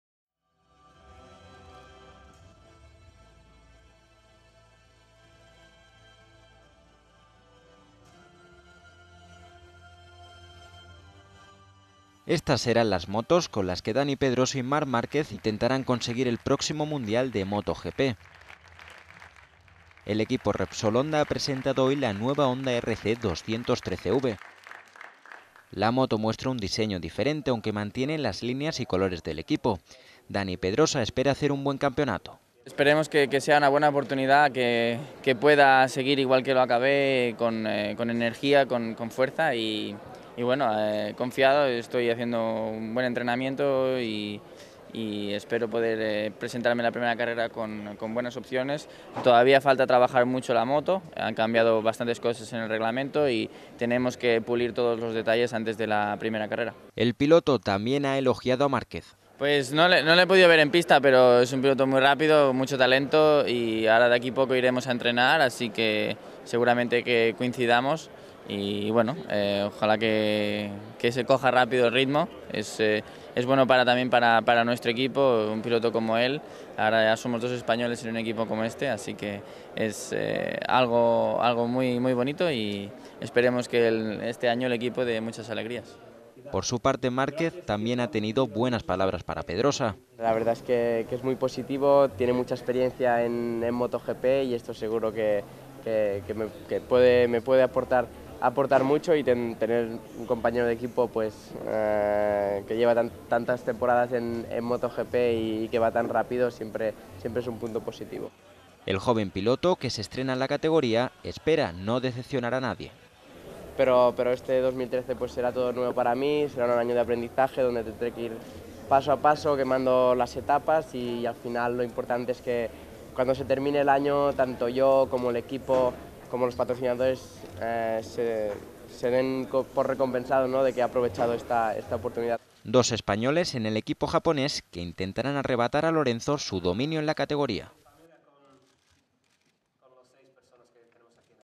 Pedrosa y Márquez presentan la Honda Repsol RC213V
Estas serán las motos con las que Dani Pedrosa y Marc Márquez intentarán conseguir el próximo mundial de Moto GP. El equipo Repsol Honda ha presentado hoy la nueva Honda RC213V. La moto muestra un diseño diferente,  aunque mantiene las líneas y  colores del equipo.